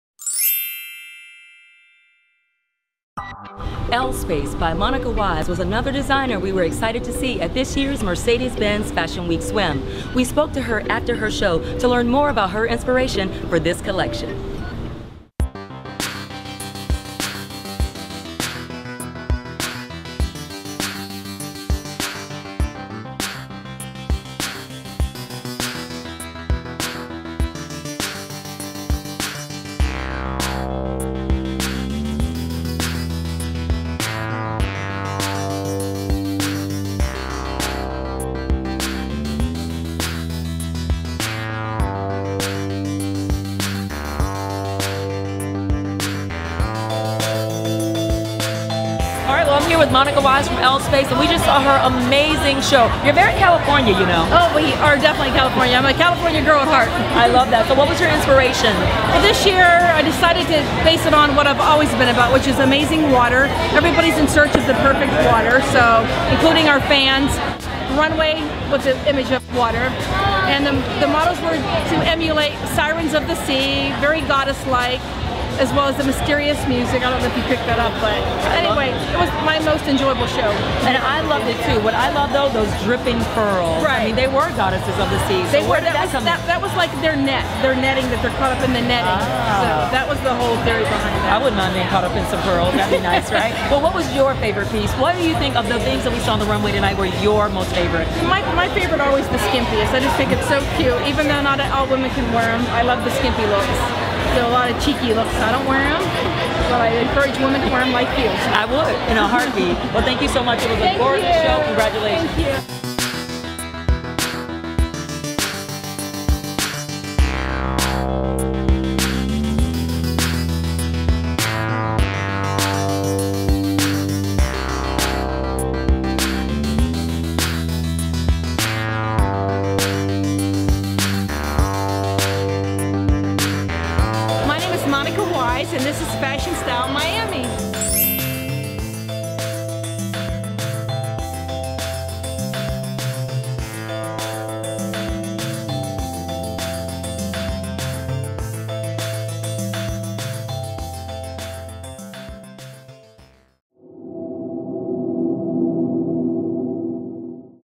FASHION STYLE MIAMI-SWIM WEEK PART 3/L*SPACE BY MONICA WISE
Host Kalyn James goes backstage to speak to designer Monica Wise about L*SPACE 2015 runway show at Mercedes Benz Fashion Week Swim.